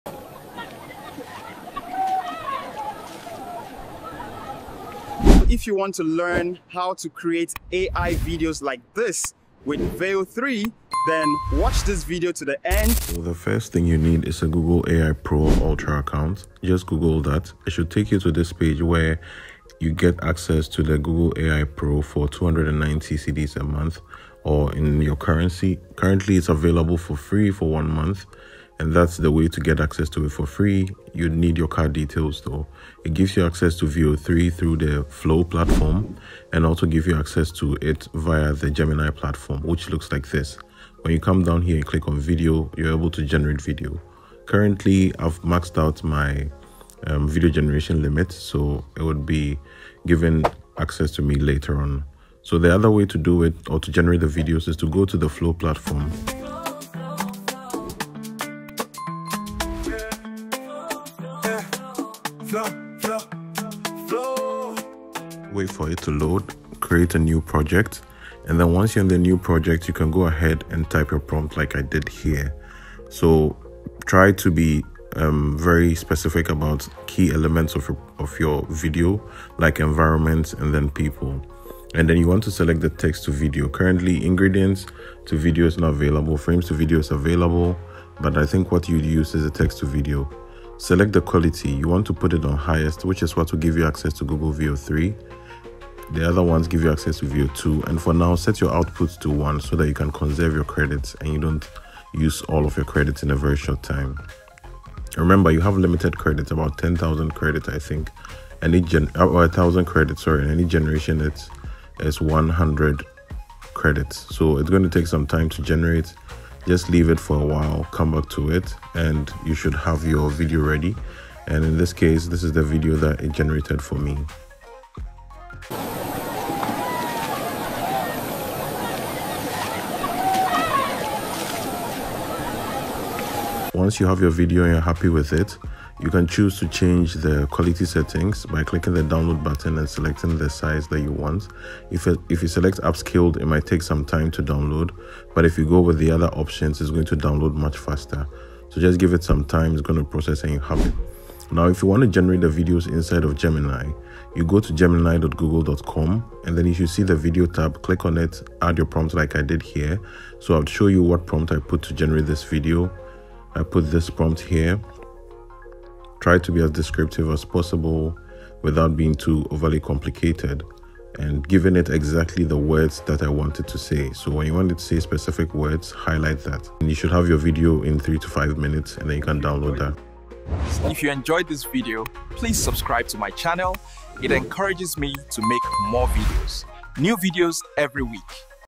Google Veo3 tutorial for beginners
In this quick tutorial, you'll discover how to access and use Veo 3, Google's powerful AI video generation tool. Whether you're a content creator or just curious about AI video, this guide will walk you through everything you need to get started with Google Veo 3 for free.